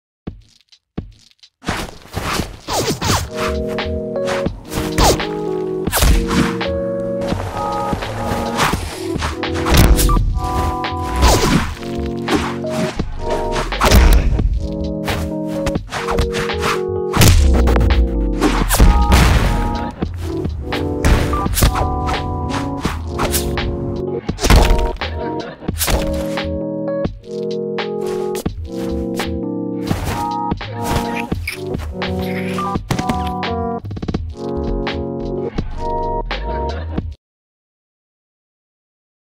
just some more practise
took about 6 and a half hours-ish. experimenting some camera angles, perspective, choreo and some of smoke and rock shit